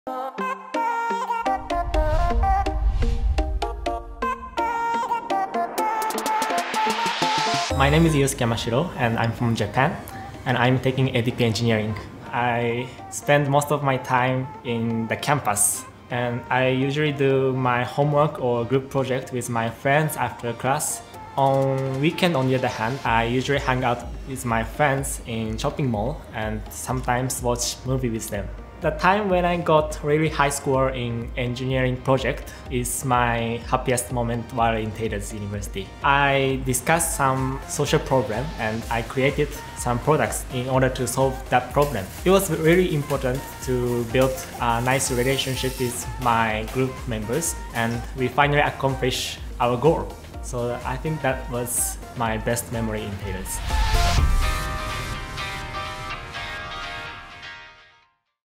Life As A Taylorian – From Our Japanese Student, Yusuke Yamashiro
Find out why Taylor’s University, Malaysia is the preferred choice for our students from Japan. From world-class campus facilities to inspiring and helpful lecturers, our students share their experiences and how they adjust to a new environment abroad.